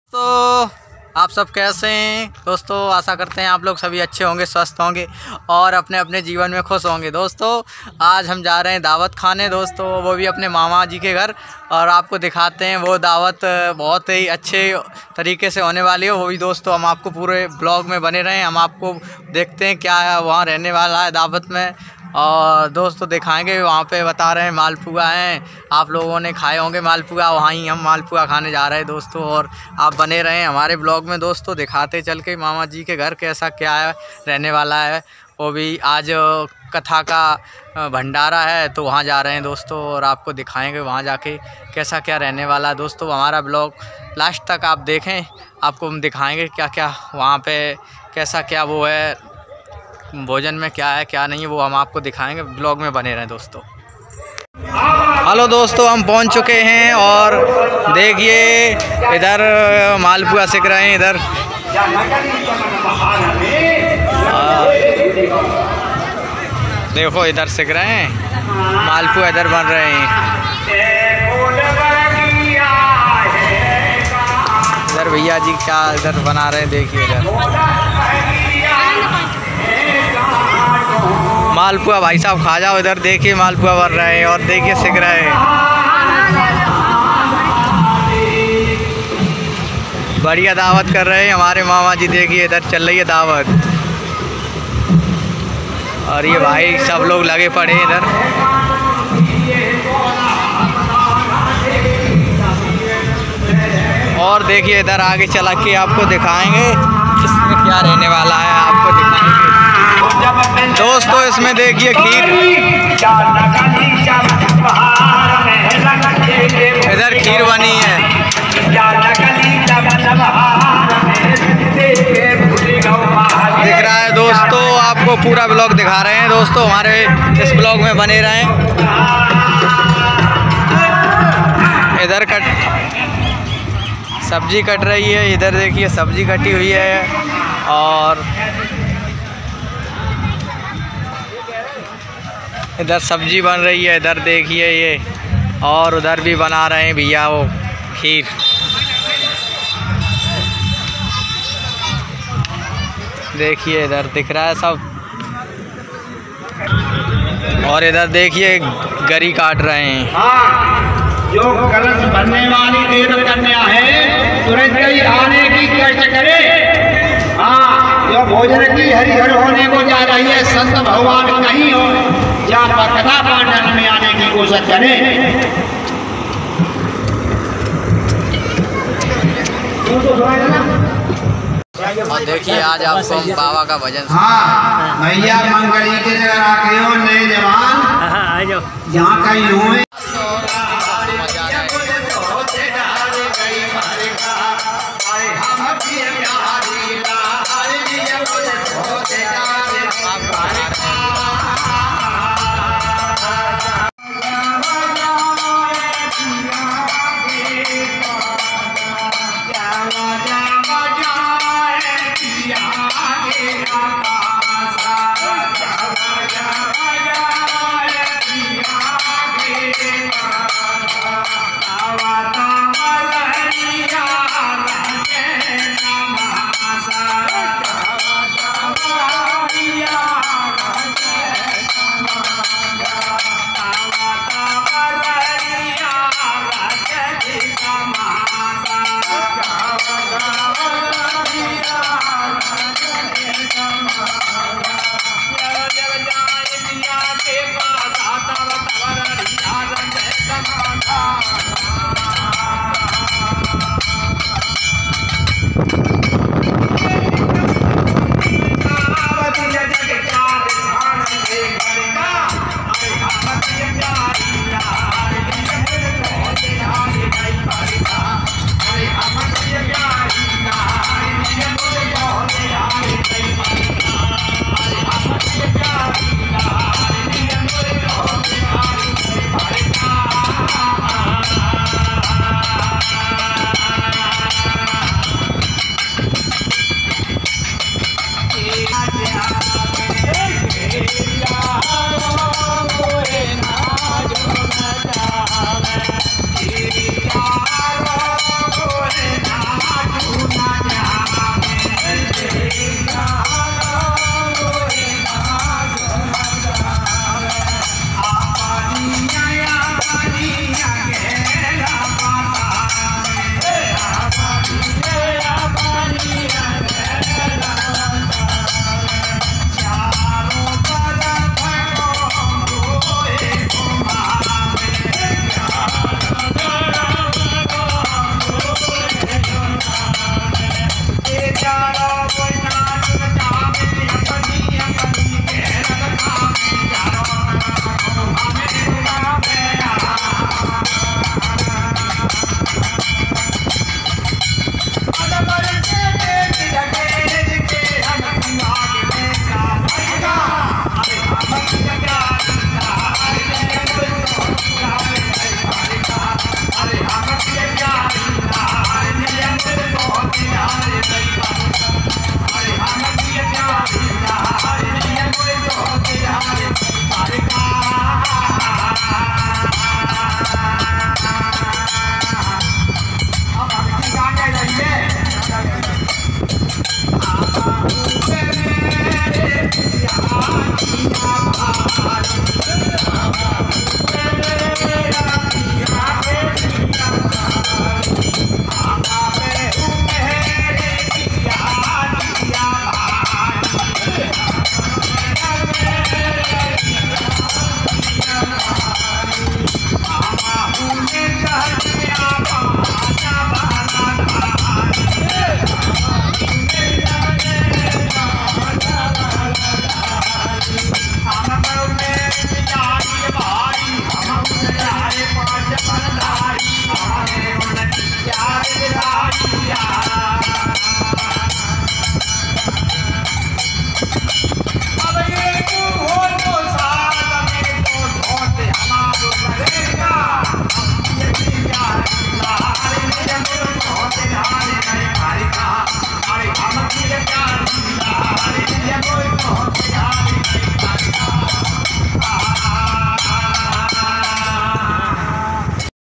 🙏शैलेन्द्र यादव गये 🌹अपने मामा के घर श्रीमत🌹 भागवत का भण्डारा खाने🌹@shailendrayadavEtah8661h
वहुत सुन्दर भण्डारा